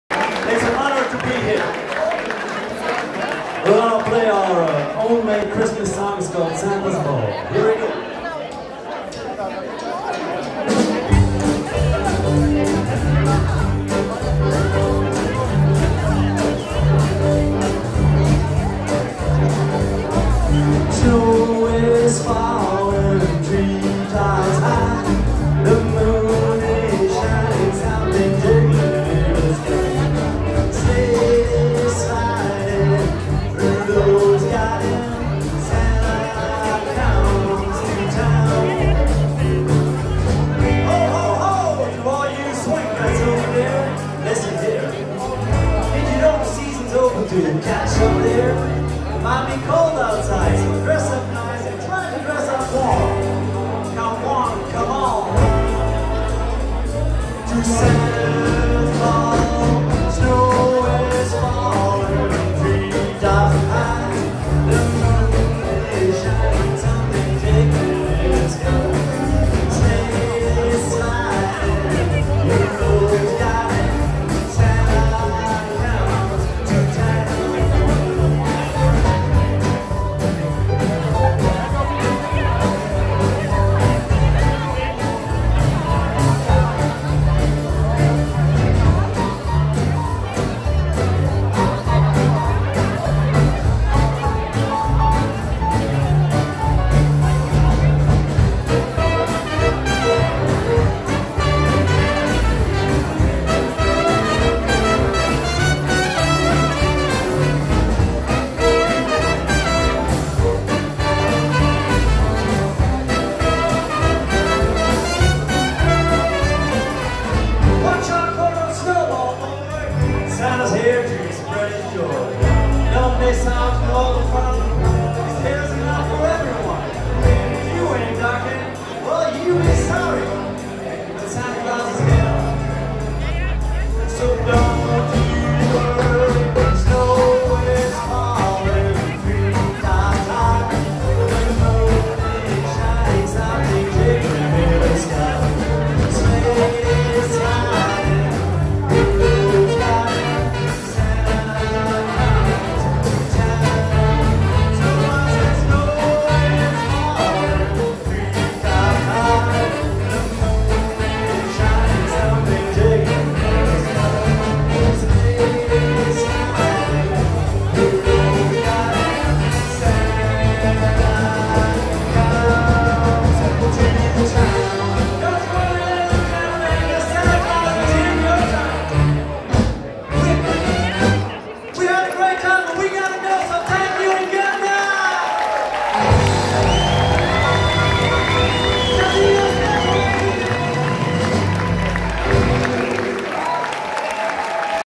SLICK NICK AND THE CASINO SPECIAL - 2 - live at Rockin' around the Christmas Tree 2019
Slick Nick and the Casino Special (BEL) perform live at Rockin' around the Christmas Tree at the Musiktheater Piano in Dortmund, Germany on December 21, 2019.